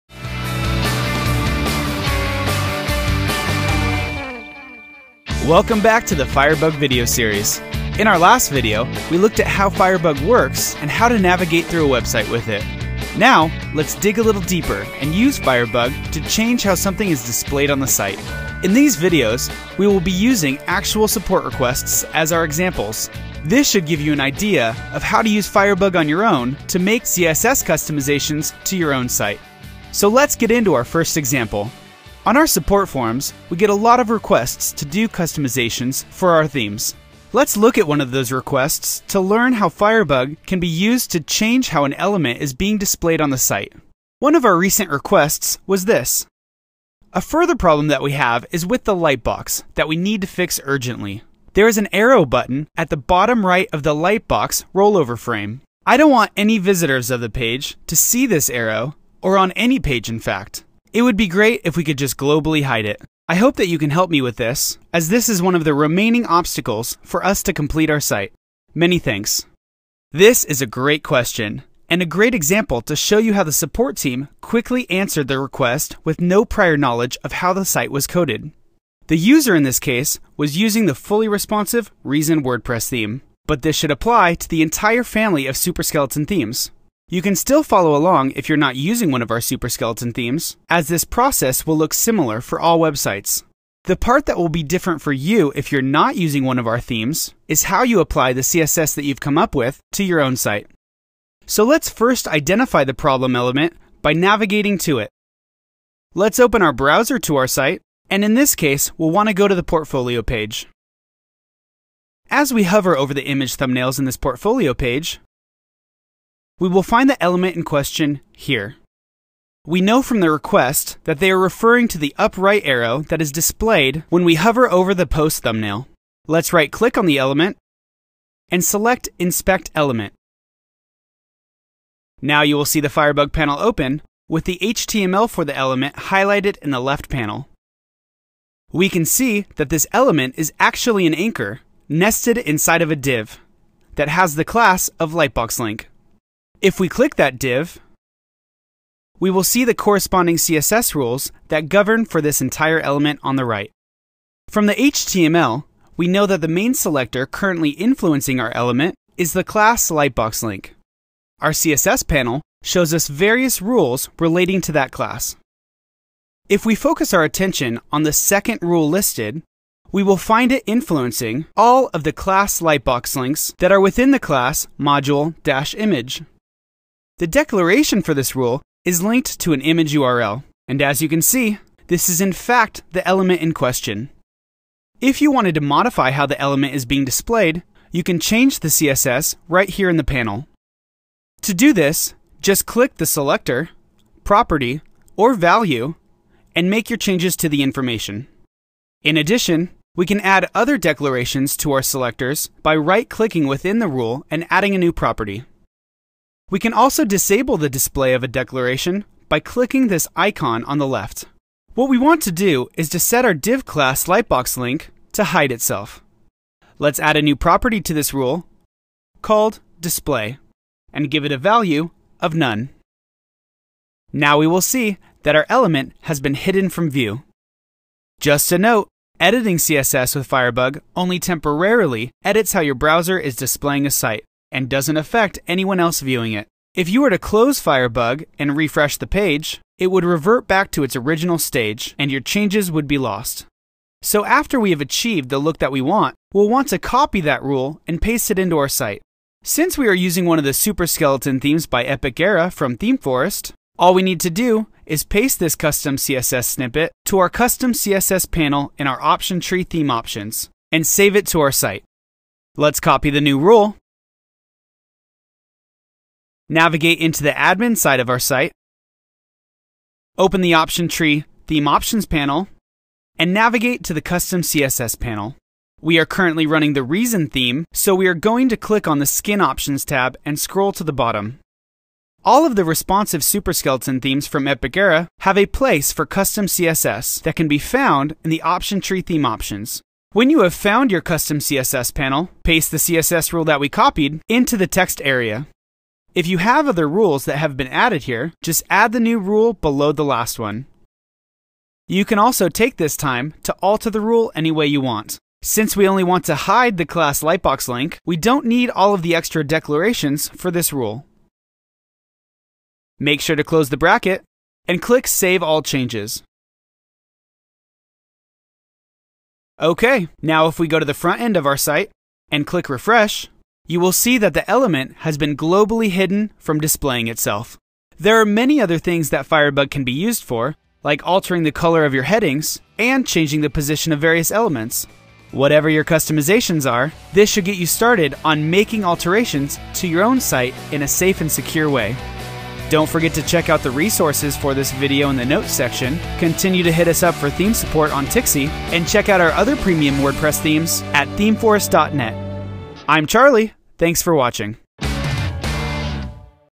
The Firebug Tutorial Series: 02 - Building Custom CSS
In this Firebug Video Tutorial we'll show you how to use firebug to change how something is displayed on the site.